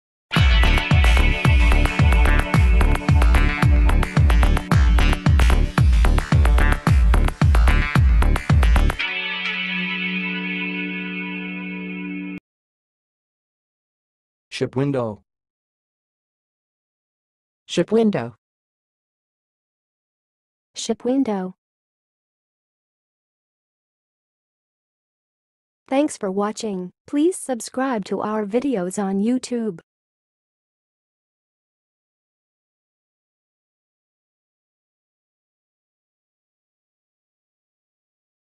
How to say "shopwindow"! (High Quality Voices)
Watch in this video how to say and pronounce "shopwindow"!